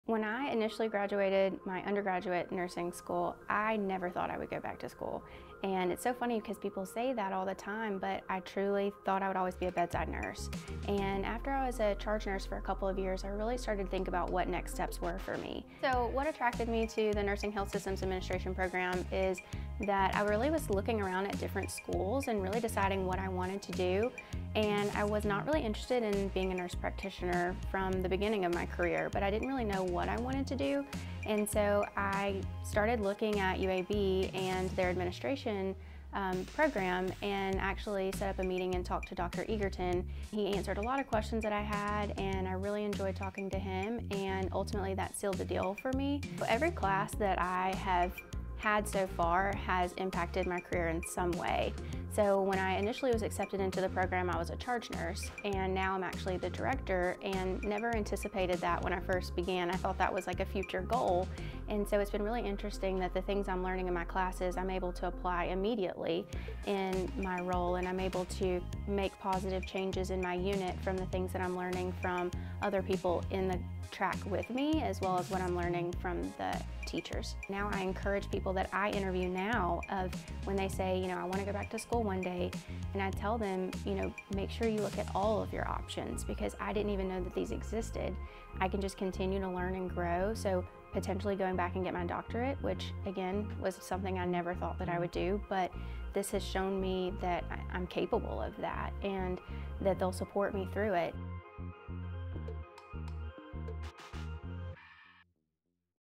NHSA Specialty is Building the Future of Nursing Administration
The UAB School of Nursing produces strong nurse leaders through it's nationally ranked programs, including the nation's No. 8 Nursing Health Systems Administration Specialty, as ranked by U.S. News & World Report for 2023. Part of the Nursing Health Systems Leadership Pathway, the Nursing Health Systems Administration specialty prepares students for leadership positions in health care systems, from unit level to system wide leadership roles. Graduates are prepared for management roles at all levels of the organization.